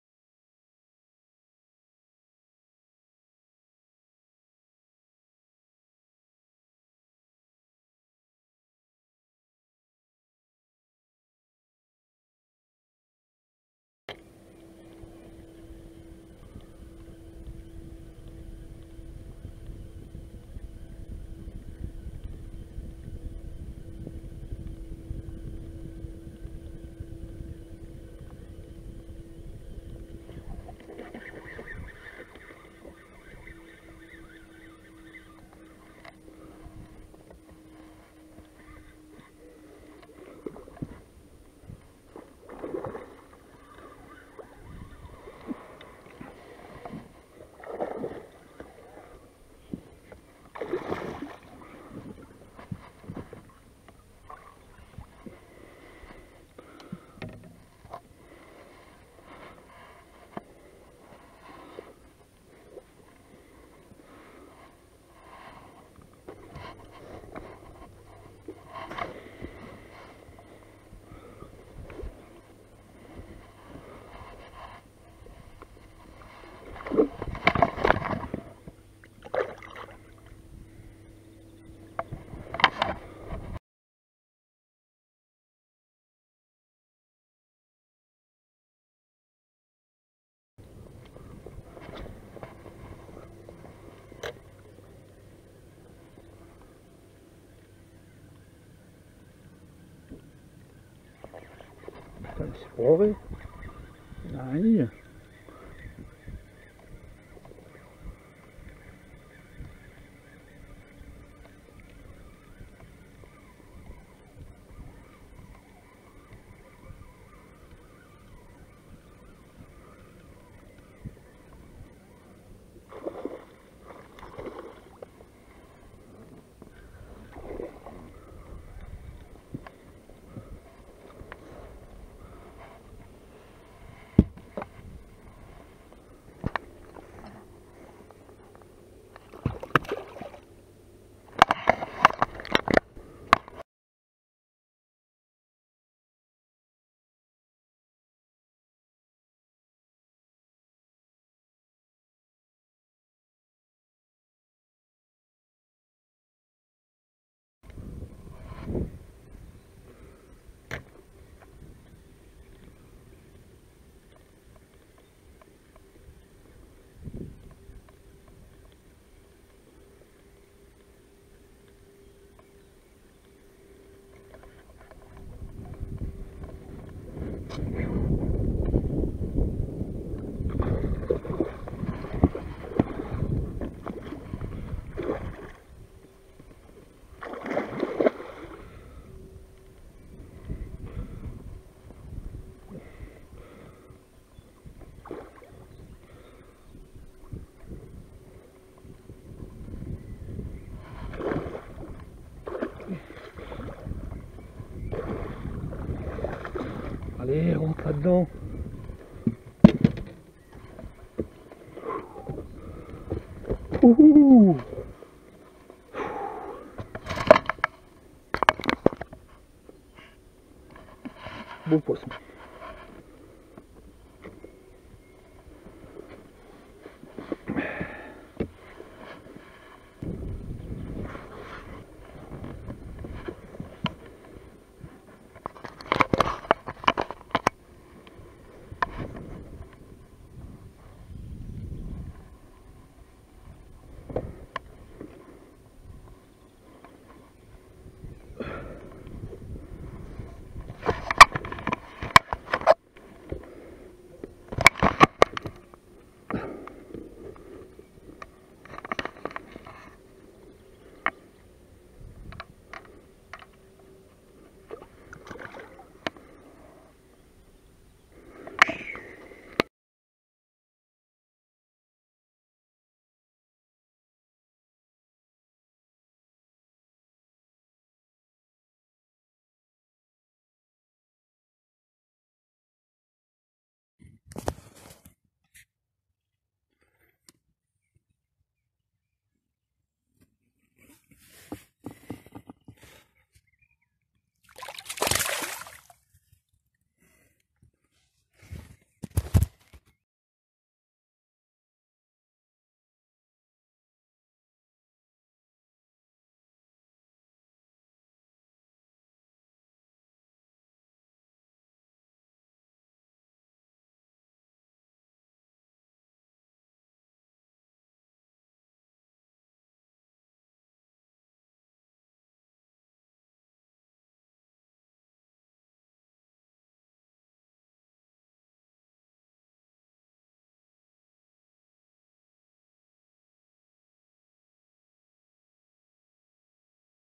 ouverture der 2019
vidéo ouverture du carnassier lac du der 2019 plusieurs brochets entre 68cm  et 86cm en no kill